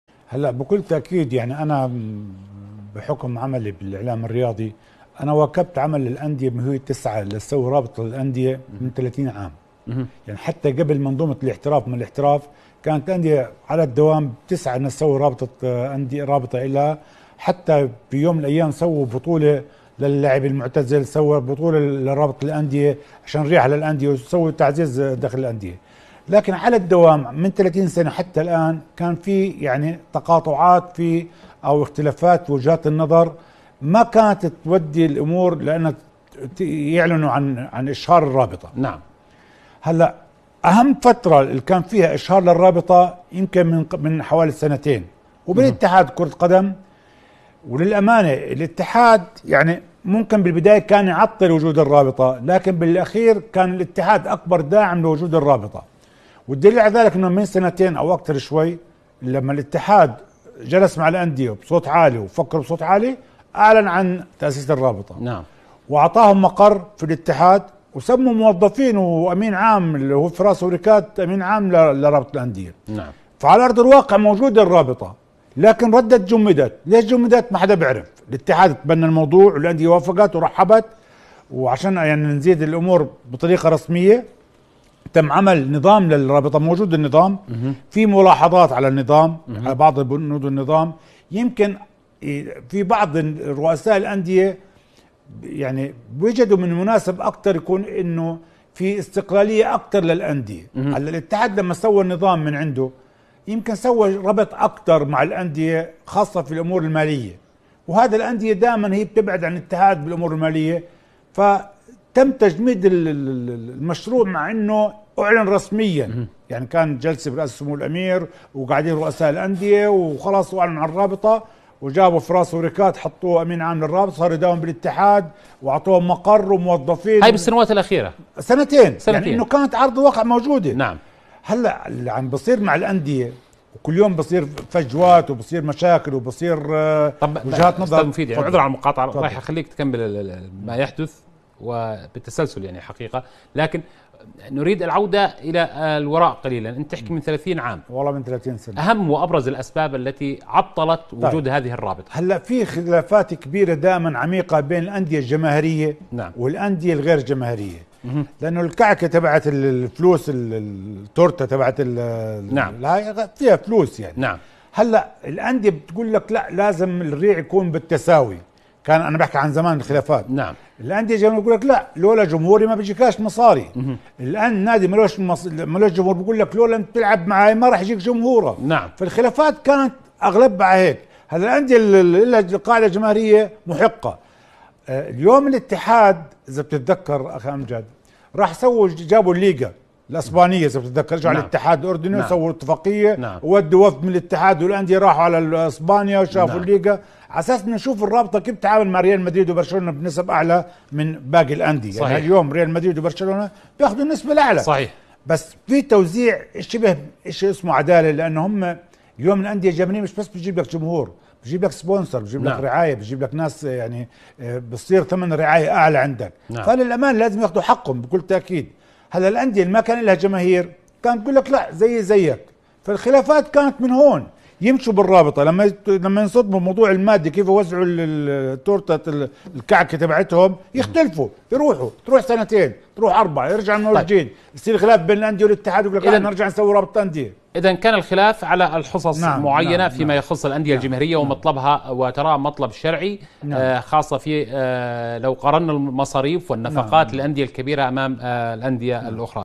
الصحفي الرياضي مفيد حسونة : من 30 عام ونحن نسمع عن تأسيس رابطة المحترفين
الصحفي الرياضي مفيد حسونة :
من 30 عام ونحن نسمع عن تأسيس رابطة المحترفين